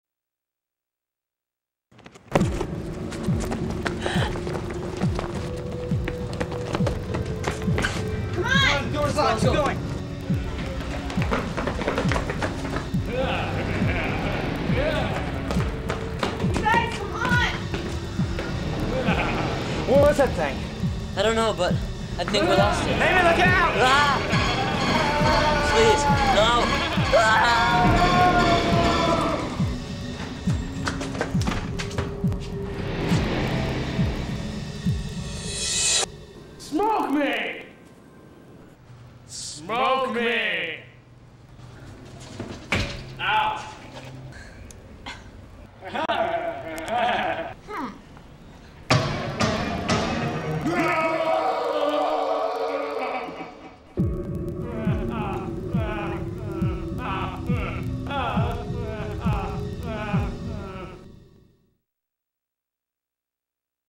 Ciggy The Cigarette
Created for the 2001 Universal Panasonic Video Competition. Actors: Ali Bloom - The Girl, Matt Bailey - Guy 1, Skyler - Guy 2, Mr. X - Ciggy The Cigarette